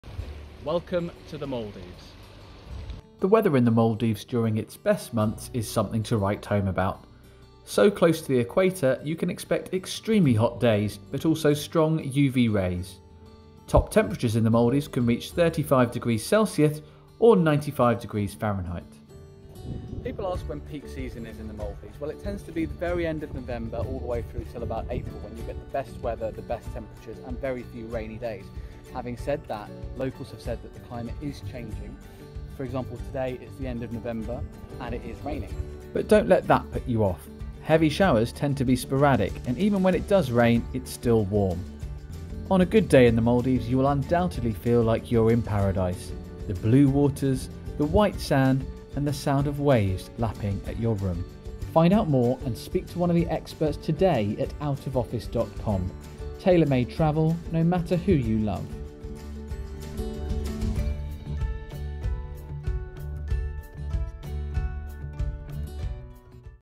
Maldives Weather - when is it rainy season?
Darren Burn from OutOfOffice.com talks you through when the best time to visit the Maldives is - from November to April. Find out more and speak to one of the team today - the luxury LGBT travel experts.